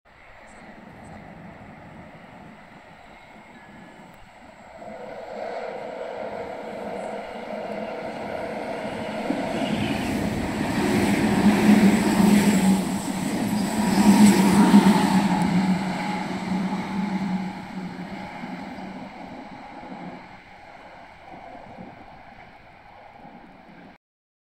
名鉄3705F+3123F 急行豊橋行き 山王駅通過
2023年3月17日撮影
編成美の急行です。3123Fは3次車唯一の赤色編成になりそうです。3120Fが赤色で舞木から出てくるかは分かりませんが、他の赤い3100系もたくさん撮りたいと思います。
また、3500系未更新、3700系も多く撮っていきたいです。